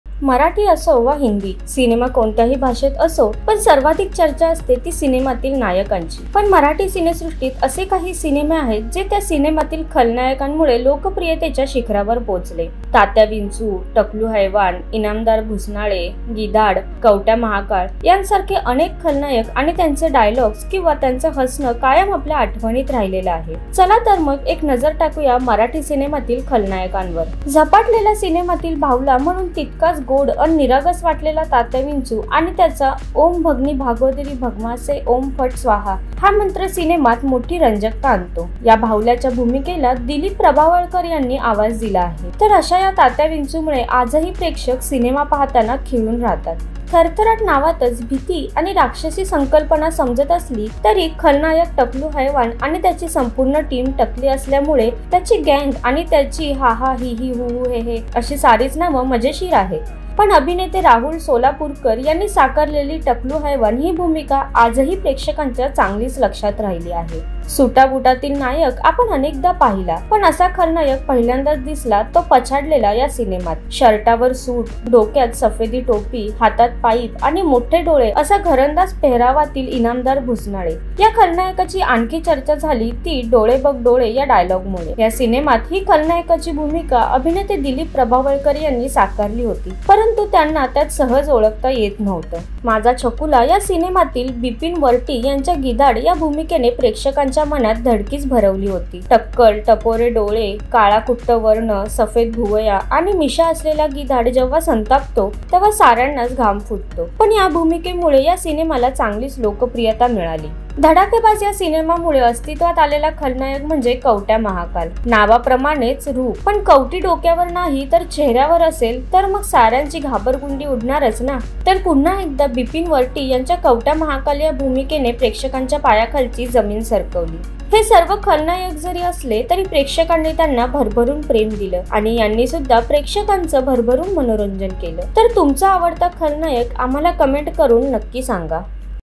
मराठी सिनेमातील लोकप्रिय खलनायक | Fillamwala
हे सर्व खलनायक जरी असले, तरी प्रेक्षकांनी यांना भरभरून प्रेम दिलं. आणि यांनी सुद्धा प्रेक्षकांचं भरभरून मनोरंजन केलं.

तर तुमचा आवडता खलनायक आम्हाला कमेंट करून नक्की सांगा.